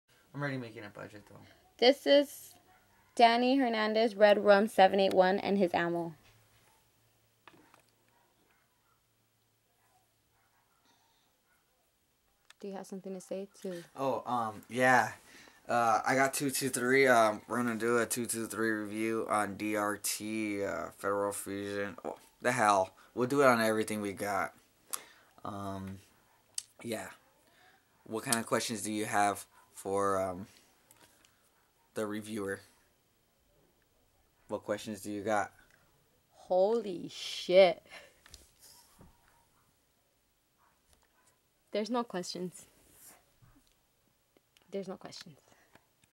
5.56 ammo testing
Just a old video I had taken along time ago. I have a much bigger variety now. Feel free to let me know if you'll like some type of ammo review and if I go it ill review it. 5.56 only